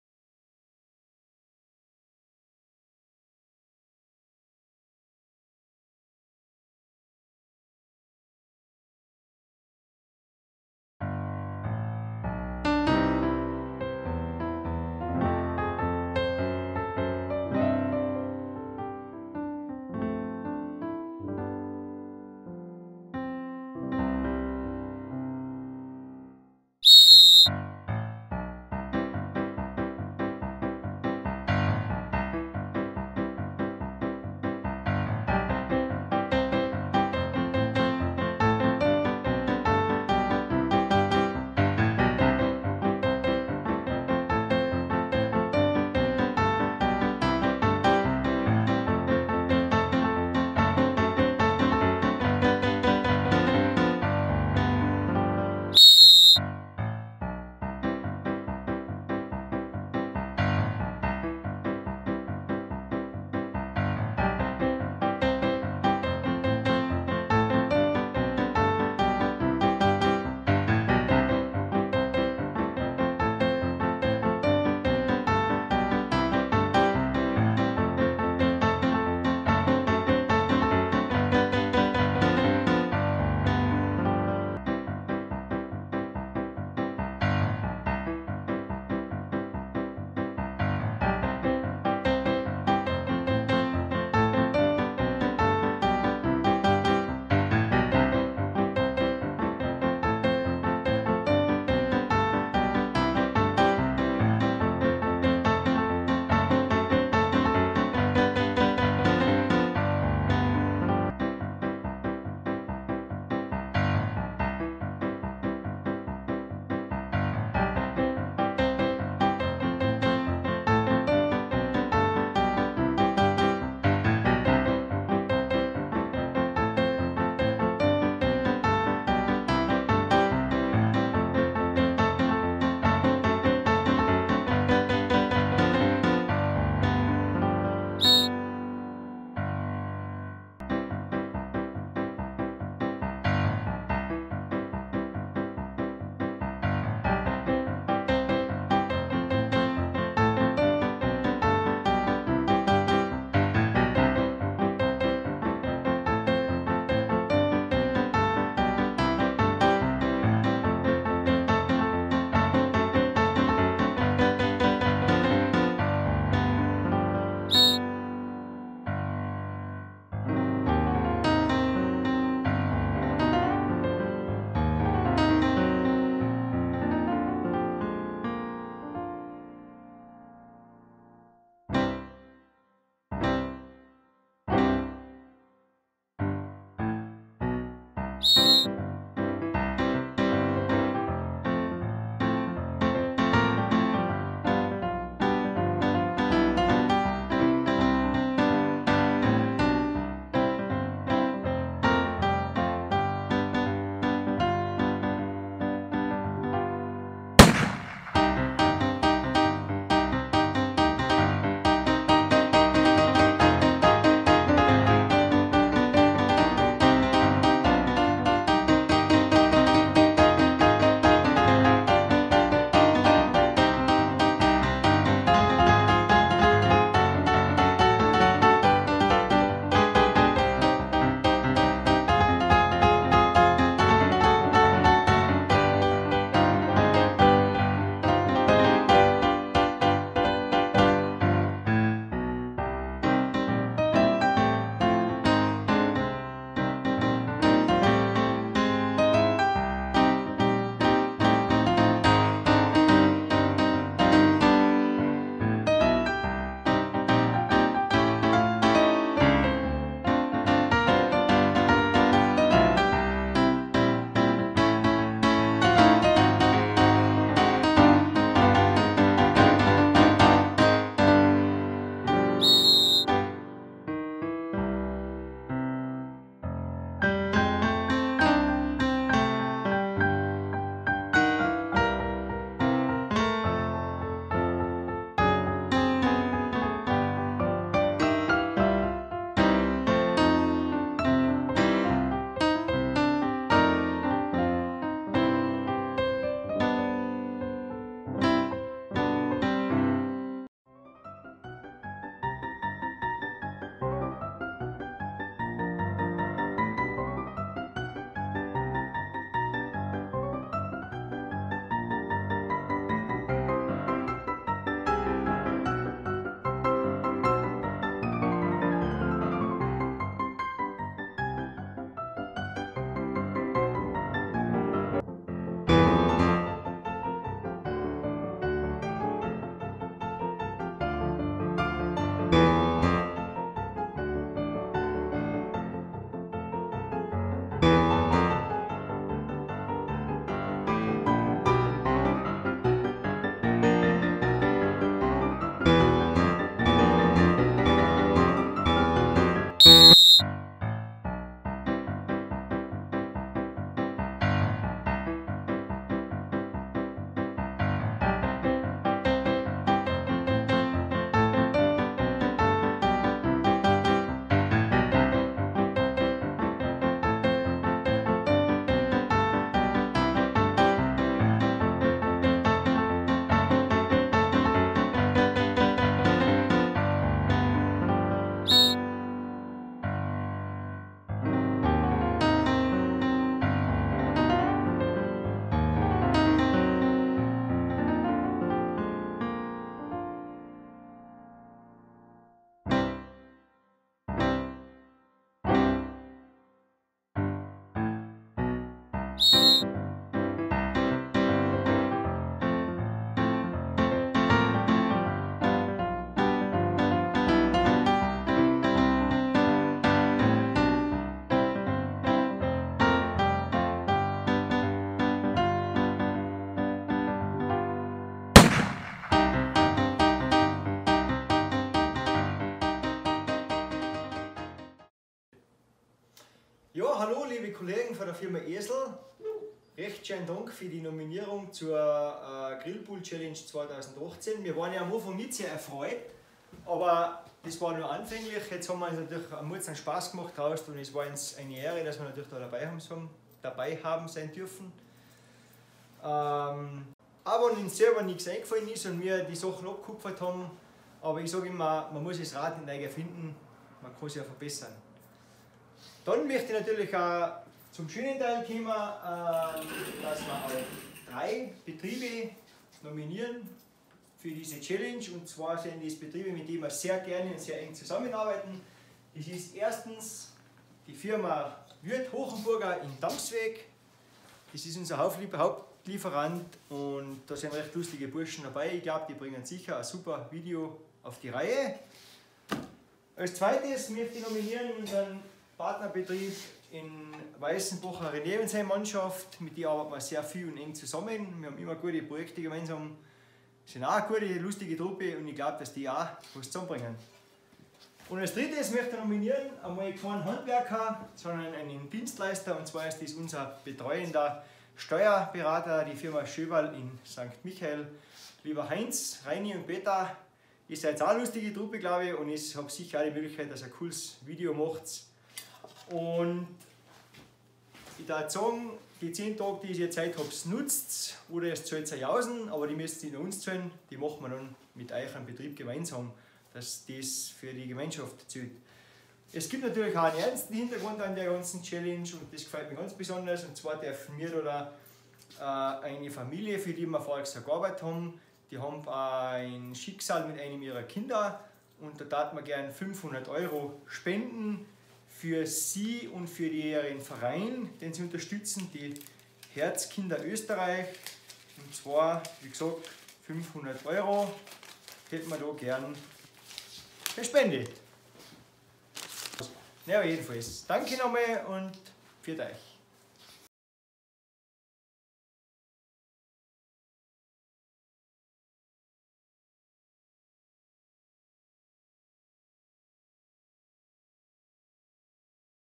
Grill Pool Challenge 2018 Steiner
steiner baukunst nimmt die grillpool challenge an und überrascht mit einem großartigem video im slapstick style.